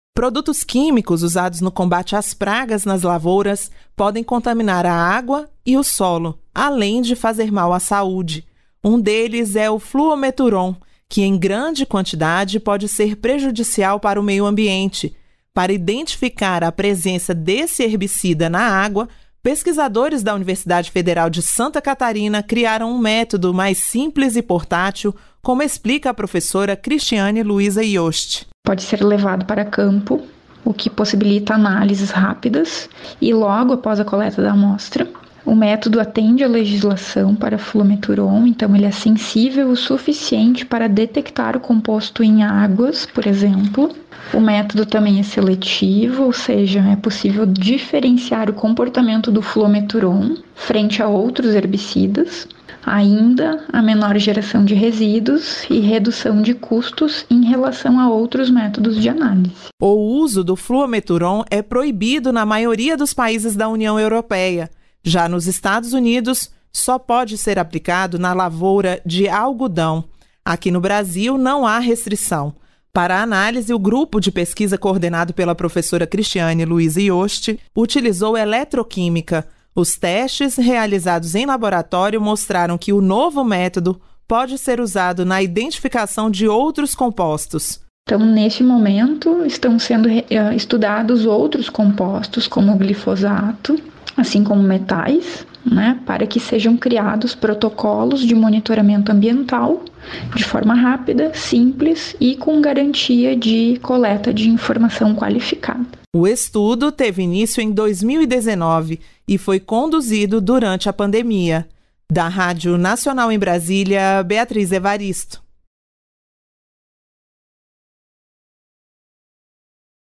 18/4/2022 PESQUISADORES DESENVOLVEM MÉTODO PARA IDENTIFICAR HERBICIDA NA ÁGUA | Ciência e Tecnologia
Método portátil, desenvolvido na Universidade Federal de Santa Catarina, é capaz de identificar altas concentrações do herbicida fluometuron na água. Em grande quantidade, composto é prejudicial ao meio ambiente.

registrado em